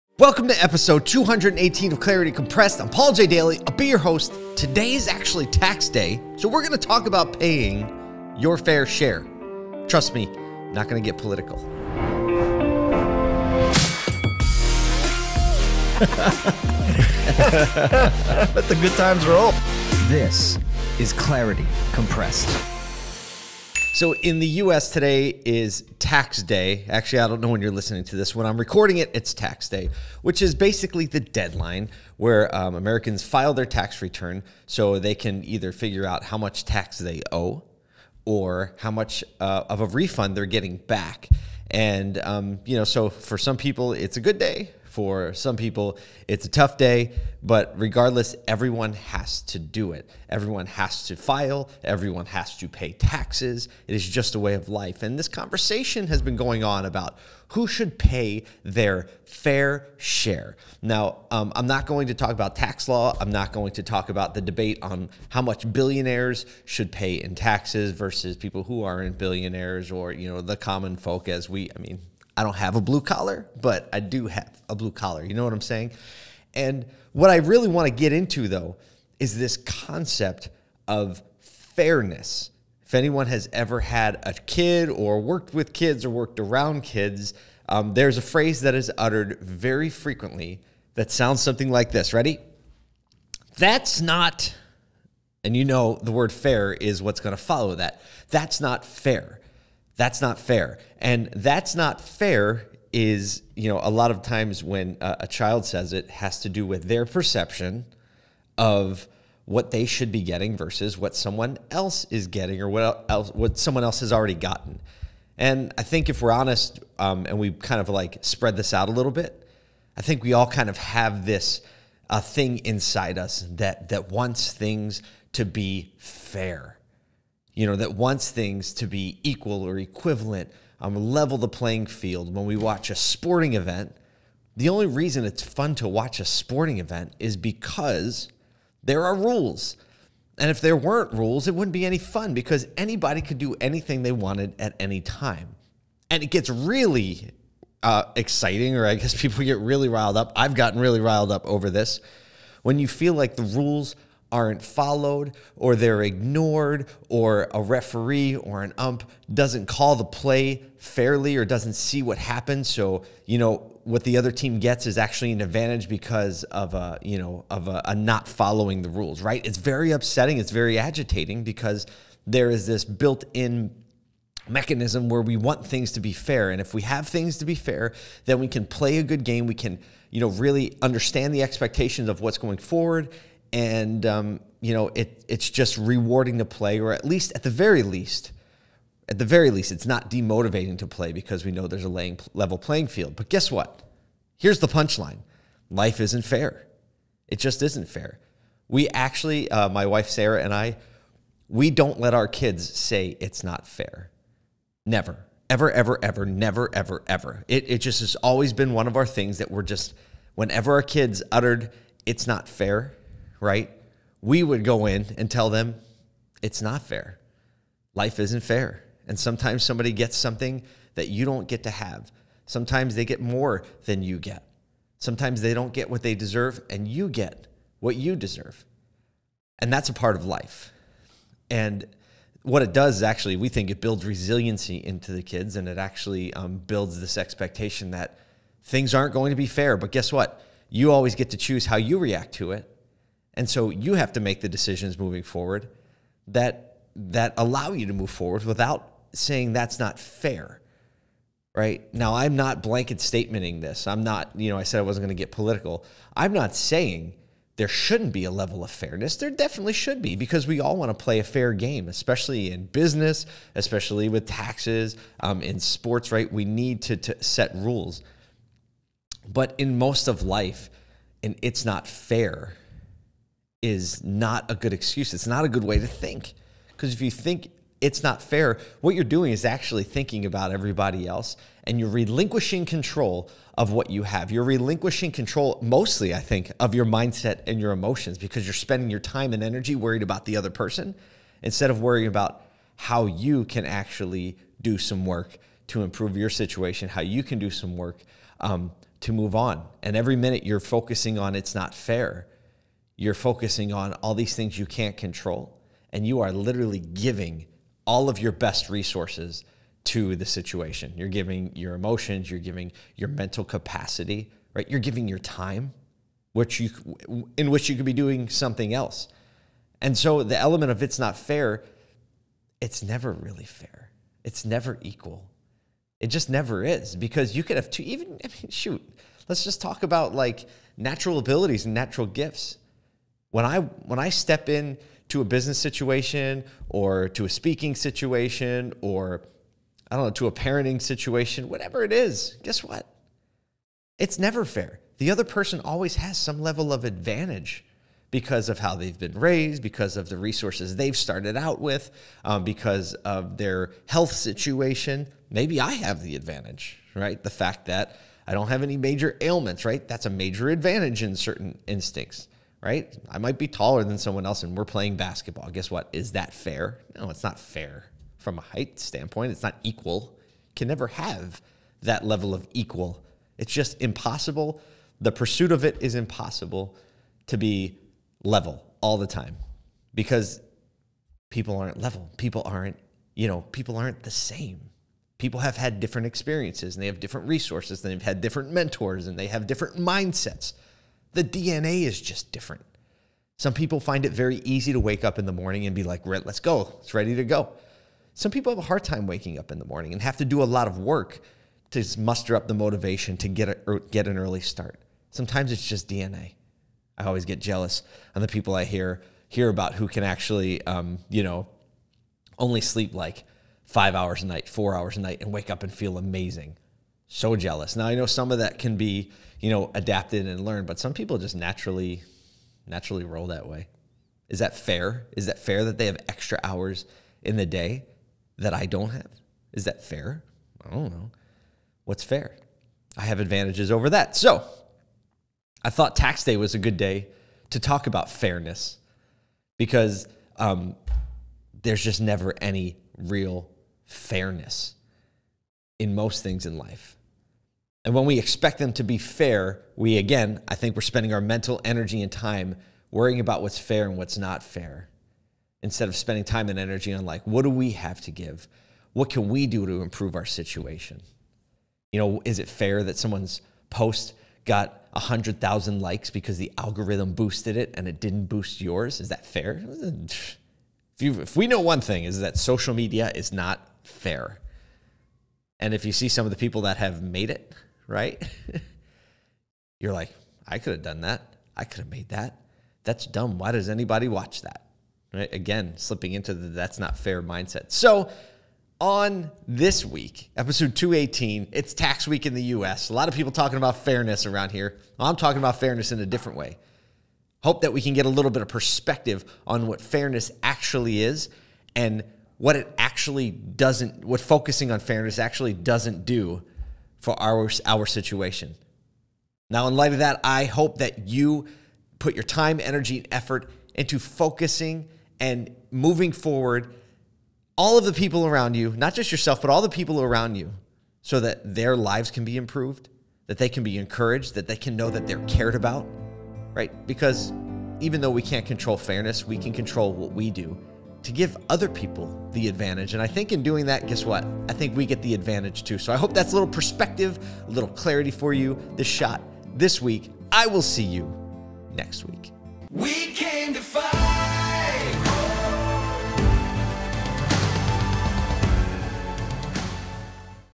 That's not fair! - Clarity Compressed: Episode 218 | Paul J Daly
Clarity Fam,

If you spent any time around young children, you are sure to hear the phrase, "That's not fair!" before long. The concept of "fair" is one that everyone wants to a point. This is especially true in sports. Without enforced rules, the game becomes less fun for everyone. However, over-focusing on 'fairness' is the quickest way to lose in so many areas of life.

The truth is that most of life isn't fair and will never be fair. This is why my wife, Sara, and I raise our kids to never use that phrase. Someone else is always smarter. Someone else is taller. Someone else has a friend in the industry to mentor them. Someone else is naturally more charismatic. Someone else has a better voice. I could go on forever.

This week I dig into the concept of 'fairness' and offer my thoughts on why I believe individuals should focus on it less.

Pursue Clarity,
Paul